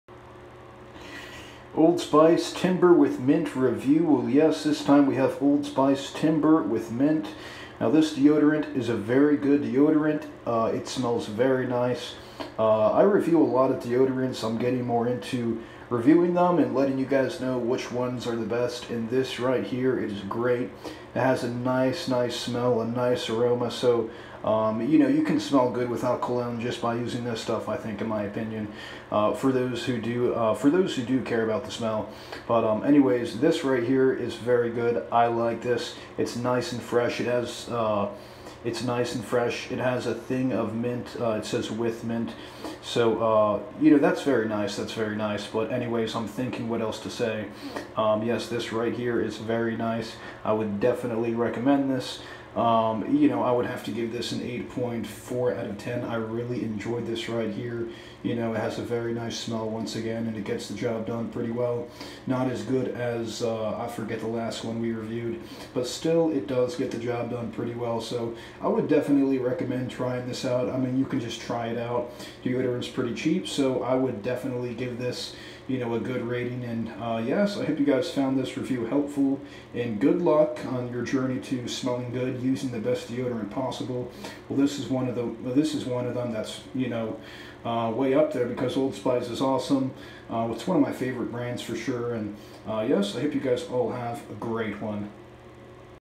Old Spice Timber (With Mint) Deodorant Review
In this video we will review Old Spice timber with mint deodorant.